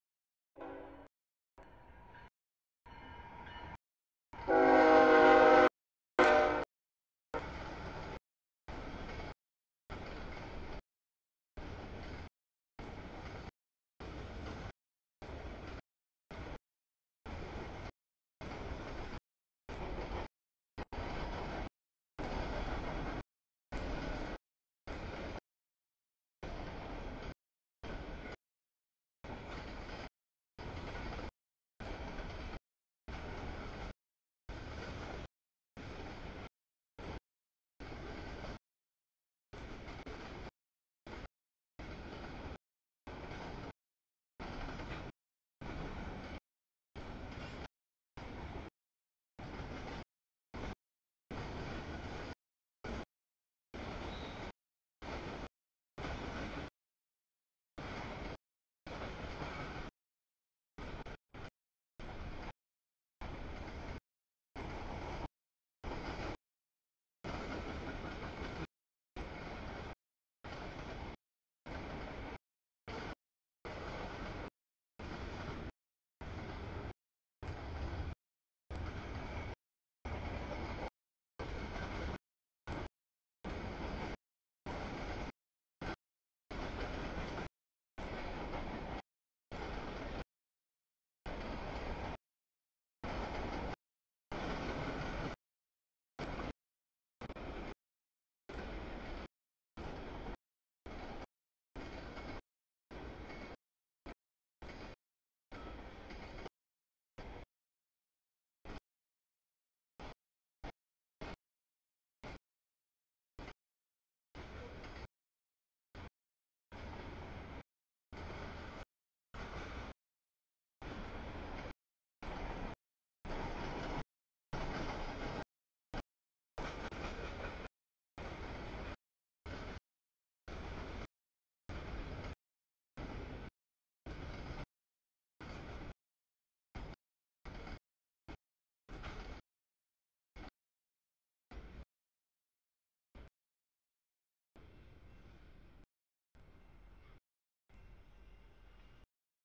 6- 14-2019 20:23 sb mix CSX 3407, 3455 Q403
00:20 sb stack CSX mDPU Q004
00:37 sb stack CSX 5435 Q162
00:55 nb stack trailers CSX 5282 Q009
02:24 nb stack CSX L167
03:16 sb mix CSX 958 mDPU Q409
03:24 nb stack CSX L159
04:08 sb stack CSX Q160
05:01 nb nysw cut, auto racks, epic trash CSX DPU X410
07:44 sb stack CSX 252, 7805 Q158
07:50 nb stack trailers CSX 3377, 593 Q003
08:01 nb stack trailers CSX 158, 5413 Q007
08:59 sb tanks CSX 999, 3333 K138
09:10 sb stack trailers CSX 5353, 529 Q010
09:27 sb stack trailers CSX Q008
09:32 nb auto rack CSX 5226 Q253
10:09 nb stack CSX 8055, 993 Q157
12:23 nb mix, epic/blue trash CSX 8822, 3397 Q434
12:47 nb IC 2466, UP 2661, CN 5671 CSX 143 K653
13:01 nb auto rack CSX 4744 Q261
15:30 sb mix, blue trash CSX 559, 510 Q433
16:18 sb mix, green trash CSX 5278, 4060 Q439
16:57 sb auto rack CSX 3201 mDPU 3258 Q234
17:08 nb mix CSX 3212, 3471, 6233 mDPU 3024 Q404
17:36 nb mix, safety tanker CSX 5212, 40 Q438
19:04 sb mix, auto rack CSX 214 Q262
20:23 sb mix CSX 3407, 3455 Q403
23:14 nb stack CSX 3043 DPU Q167